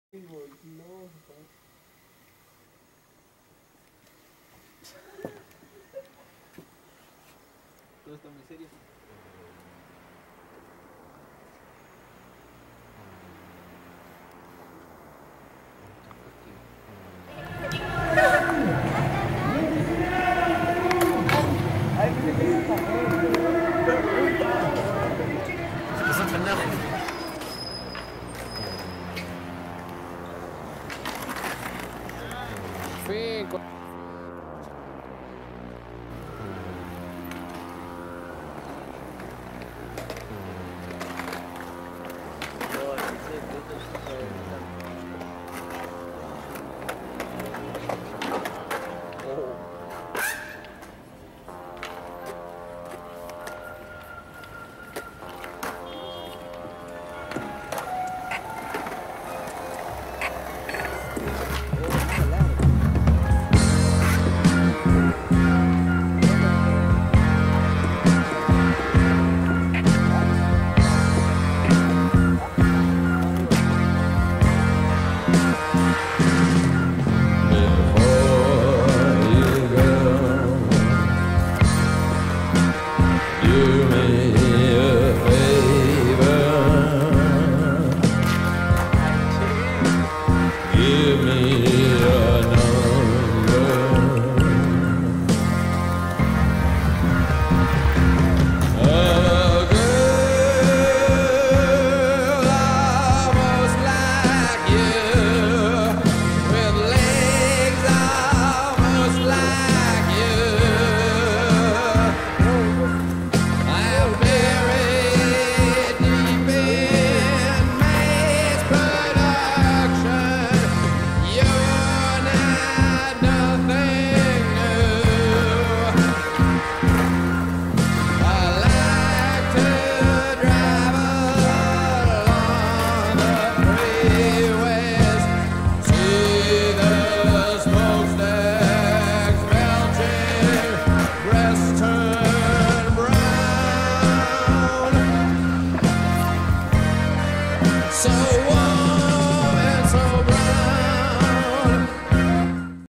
IRRE$PN$BLE
Disfrutando un poco de skateboard en la ciudad heroica
TACNA-PERÚ